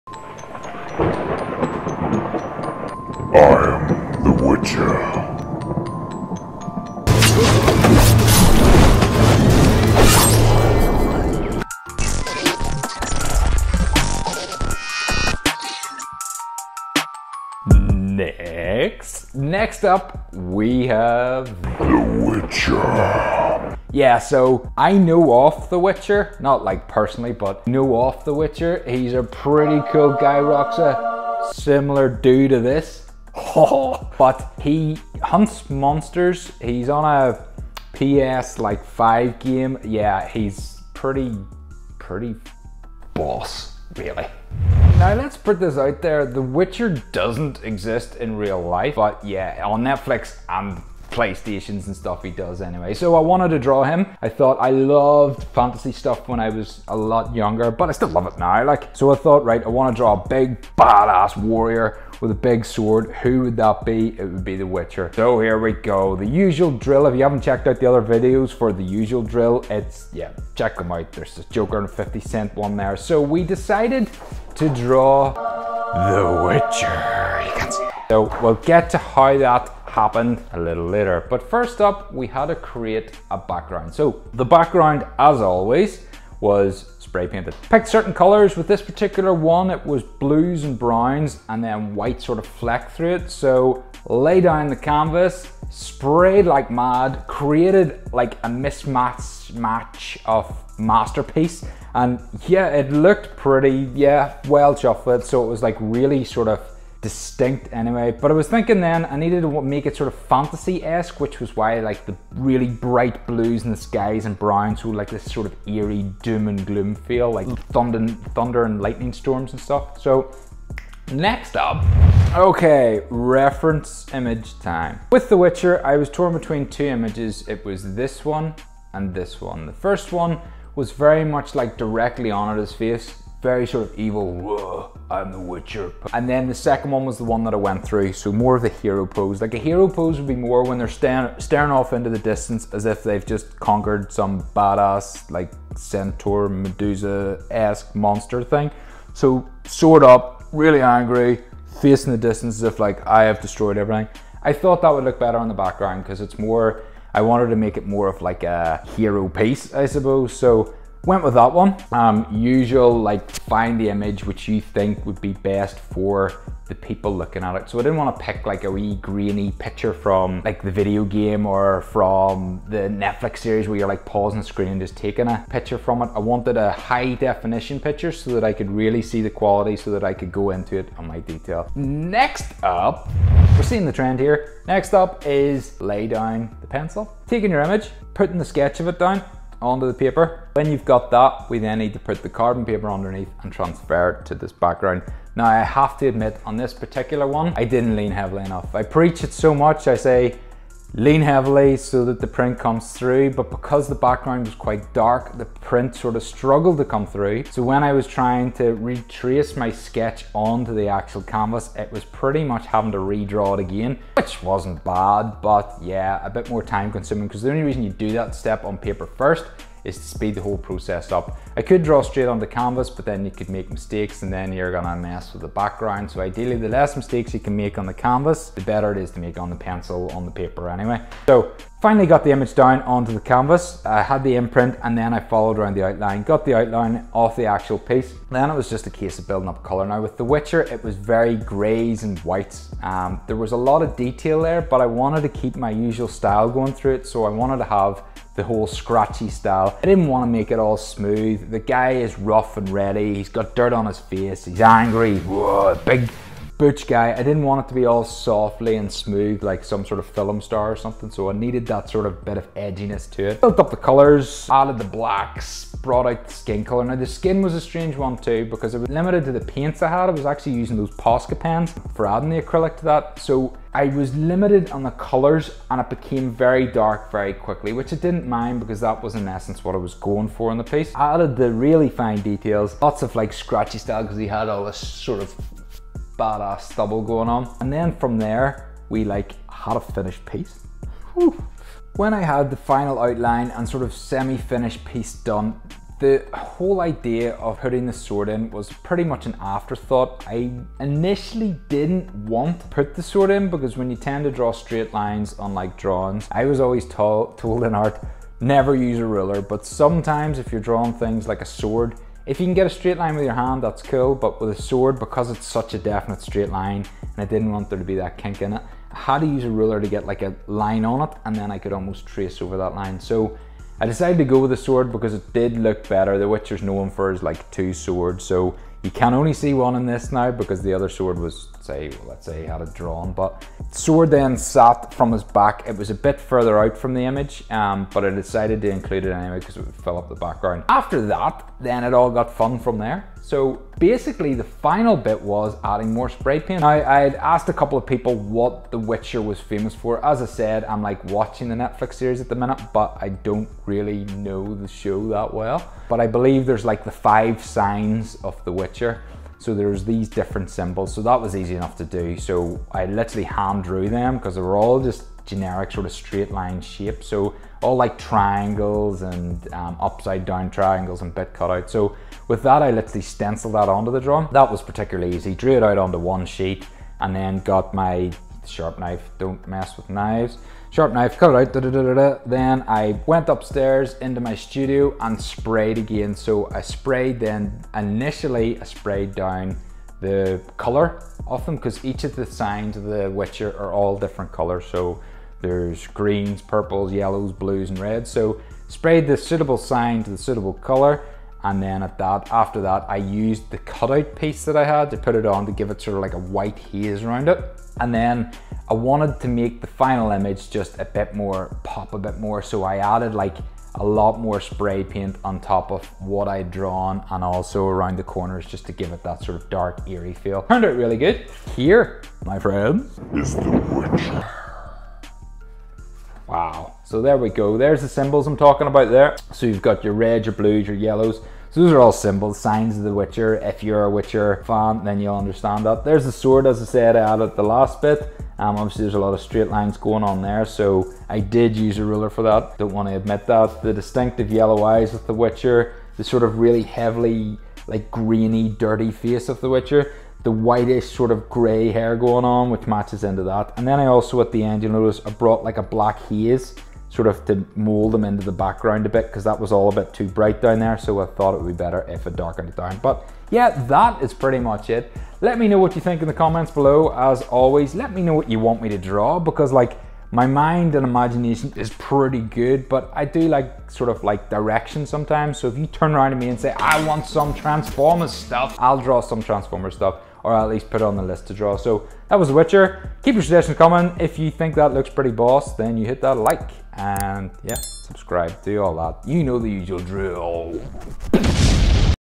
How to draw the Witcher step by step
The Witcher? You saw it? You know the cool bloke with the hair that kills monsters? Yeah, the witcher was a must draw for me. SO I have detailed a How to draw the Witcher step by step guide.

Let me know how you get on.

0:00 - 1:13 Intro
1:14 - 5:39 How to draw
5:40 - 8:12 Finishing touches

Who would you like me to draw next?